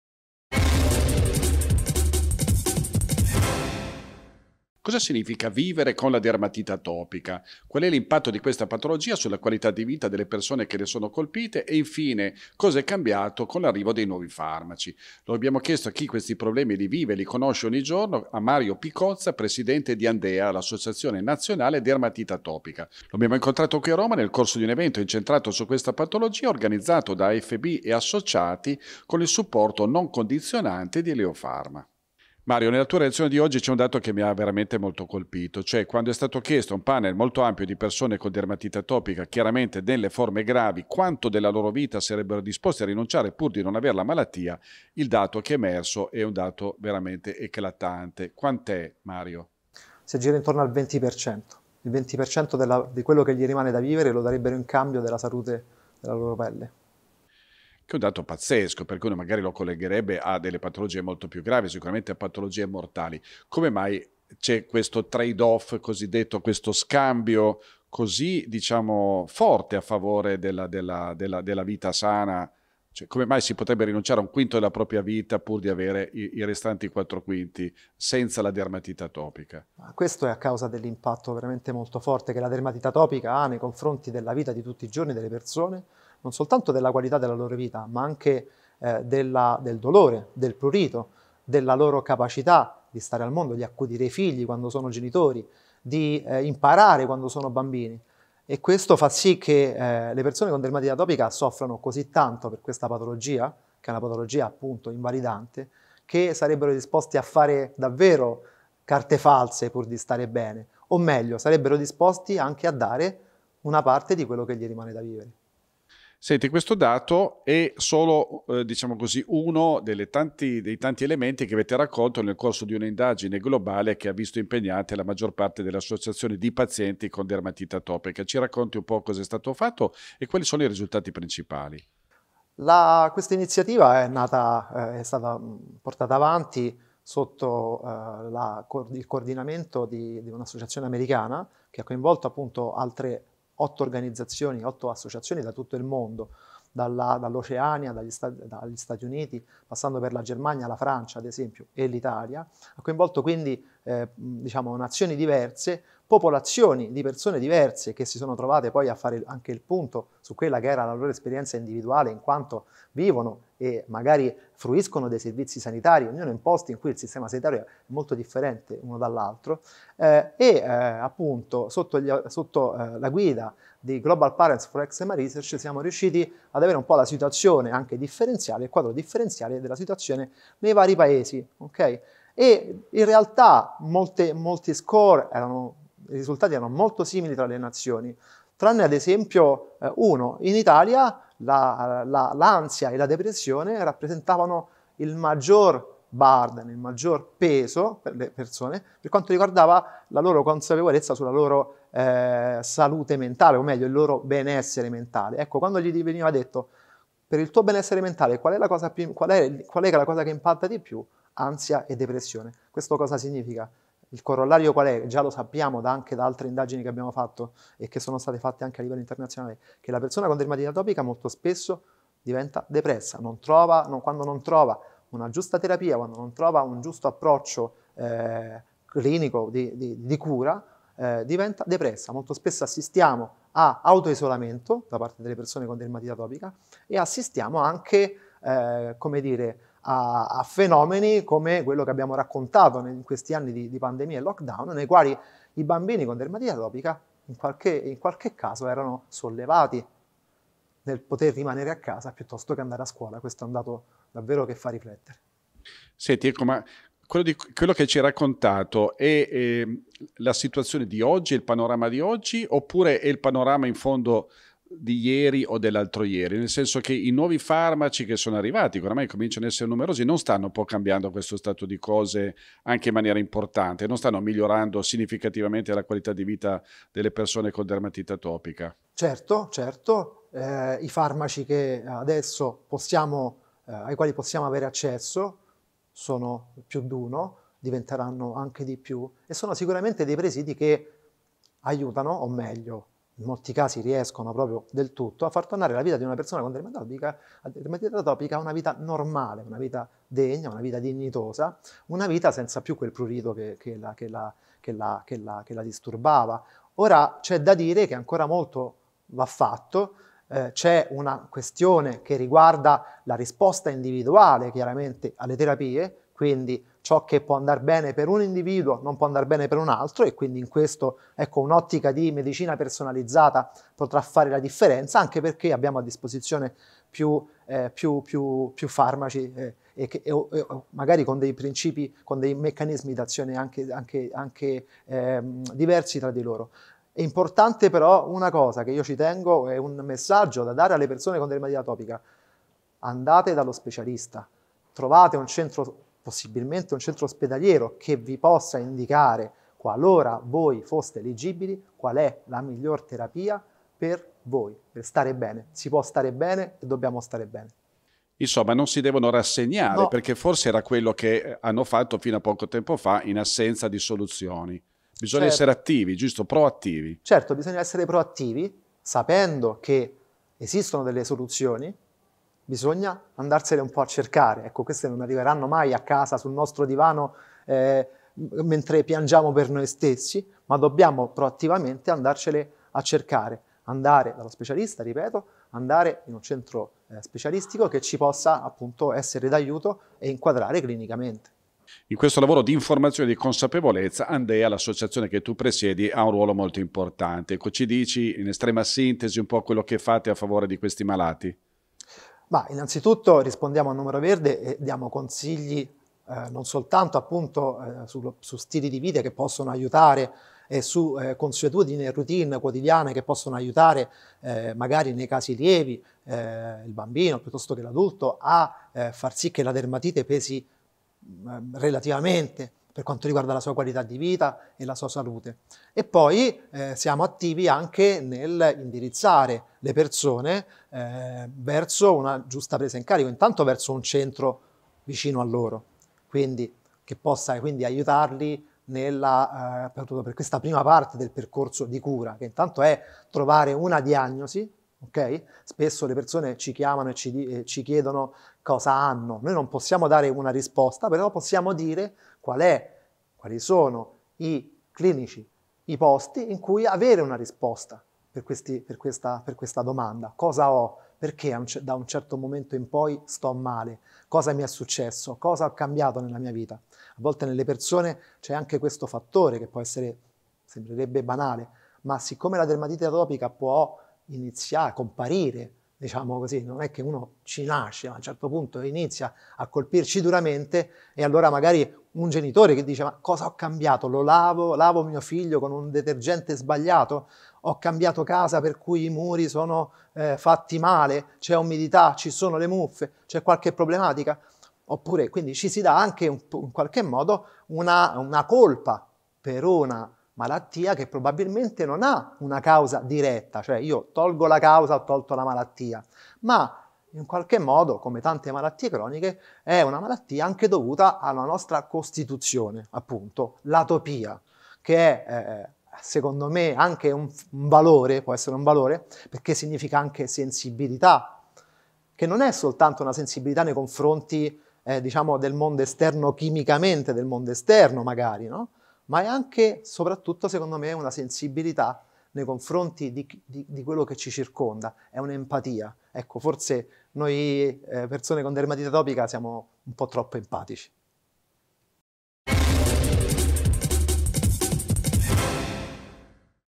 Dermatite atopica: impatto sulla qualità della vita e cosa cambia con le nuove terapie
Le persone con dermatite atopica grave sarebbero disposte a rinunciare al 20% della loro vita in cambio della salute della loro pelle. Questo dato eclatante consente di valutare l’impatto di questa malattia, considerata spesso erroneamente “non importante”, per il paziente. Ciò è probabilmente dovuto all’impatto che questa malattia ha sulla vita di tutti i giorni, per il dolore e il prurito, ma anche per la capacità di svolgere le azioni quotidiane, di accudire alla famiglia e ai figli, di studiare. Questo dato emerge da un’indagine svolta dalle associazioni dei pazienti con dermatite atopica in Italia e nel mondo. Di questa iniziativa e dell’impatto delle nuove cure sula qualità della vita parla ai microfoni di PharmaStar il Dr Mario Picozza, Presidente ANDeA, Associazione Nazionale Dermatite Atopica, ai margini dell'evento "Oltre la pelle: Il vissuto del paziente con dermatite atopica", organizzato da FB&Associati e supportato da Leo Pharma, svoltosi a Roma.